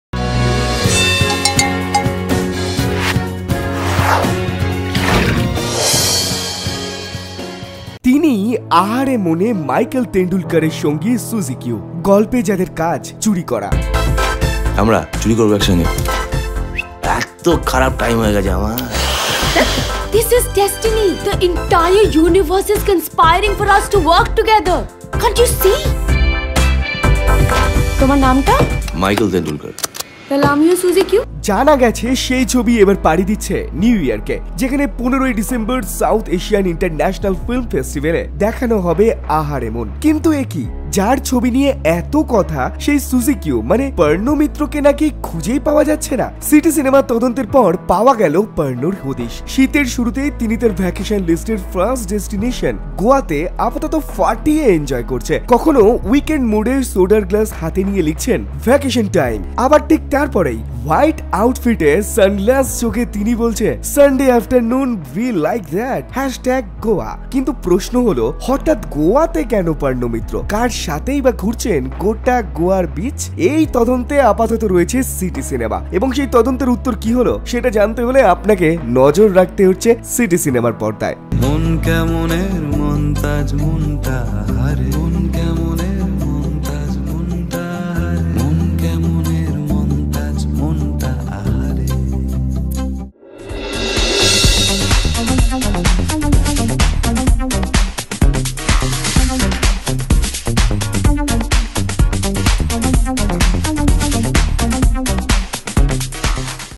Why is Parno Mittra in Goa ? | পার্ন কেনো গোয়ায় ?
The most popular actress of Tollywood Parno Mitra is in Goa right now. To know why check this video out only on Siti Cinema